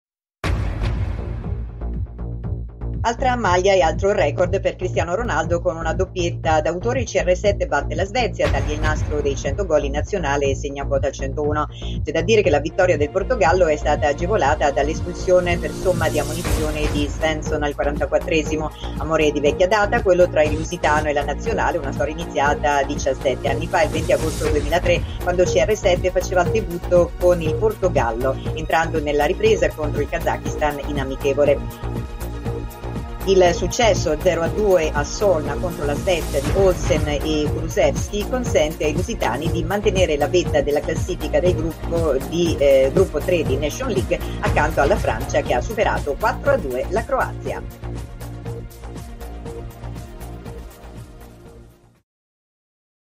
Nations League: vittoria e record per CR7 con il Portogallo
Altra maglia ed altro record per Cristiano Ronaldo. Con una doppietta d’autore CR7 batte la Svezia, taglia il nastro dei 100 gol in nazionale e segna quota 101.